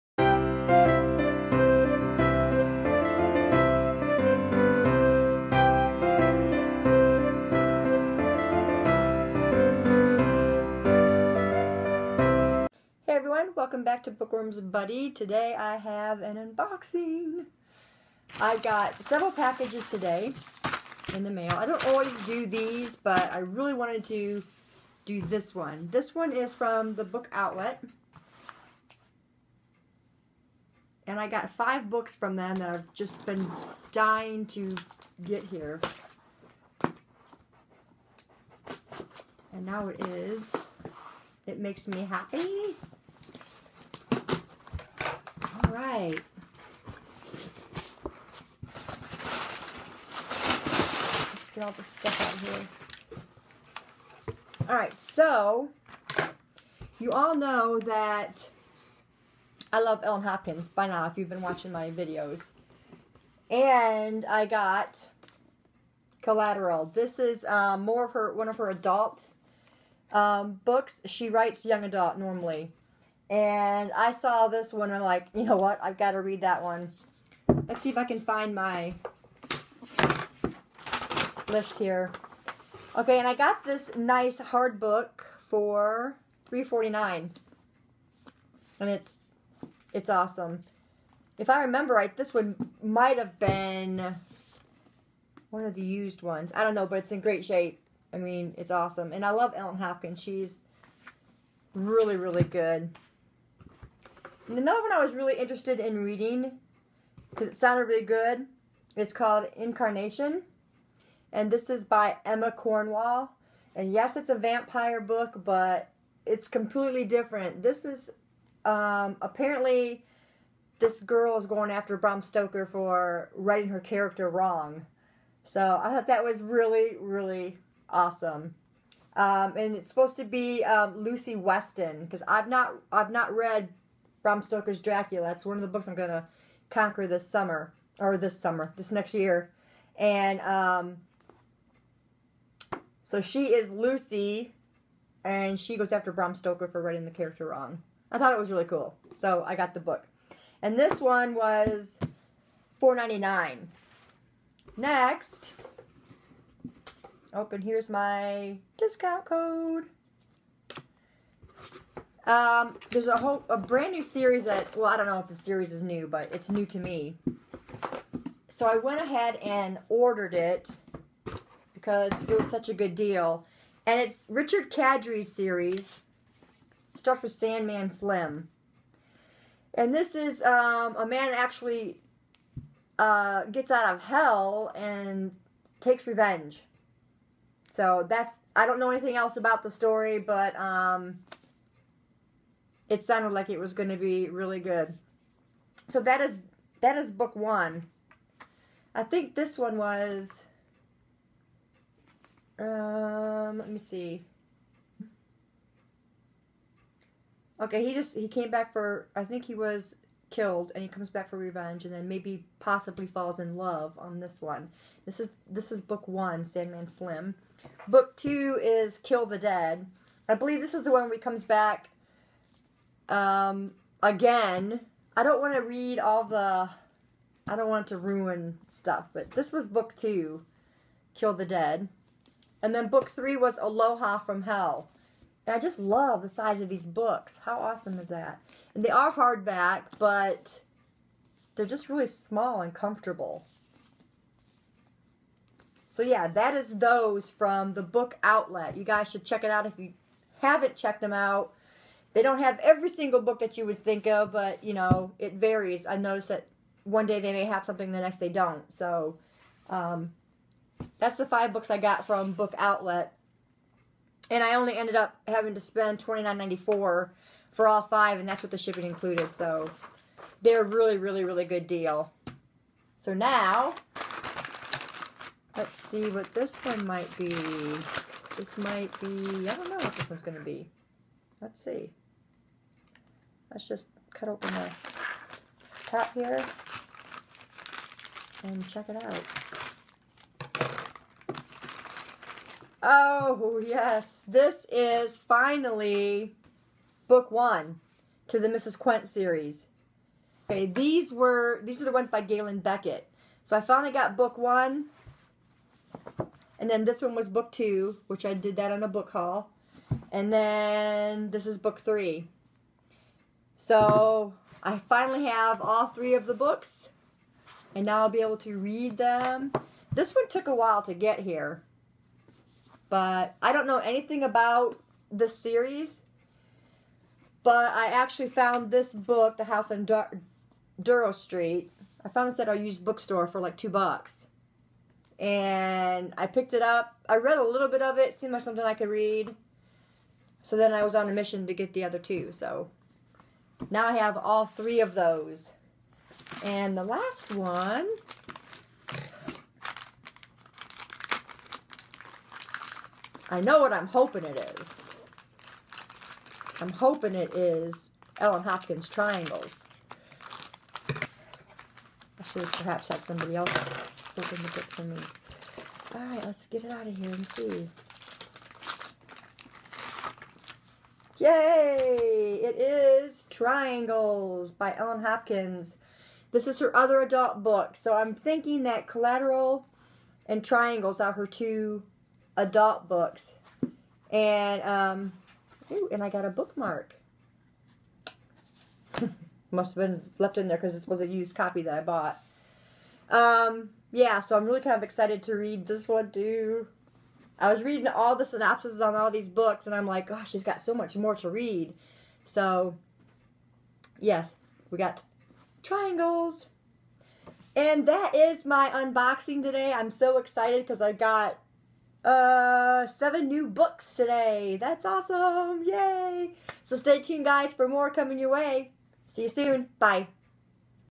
We have our first UNBOXING!
Yay so excited that I got to do my first unboxing for everyone! I have patiently been waiting for these books to come! And yay, here they are!